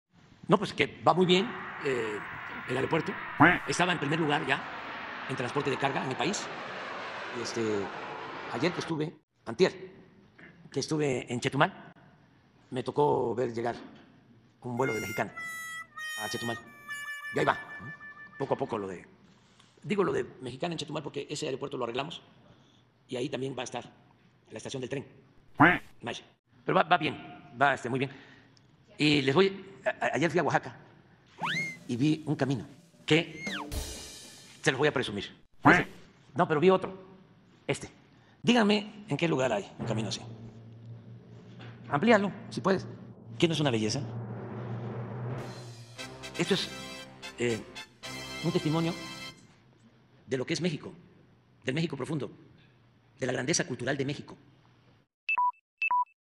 Va muy bien el AIFA, pero lo que les quiero presumir son los caminos de Oaxaca 🦆
Va muy bien el AIFA, estaba en primer lugar en transporte de carga en el país, vi que un avión de una aerolínea japonesa había llegado al AIFA, pero lo que les quiero presumir son los caminos de Oaxaca 🦆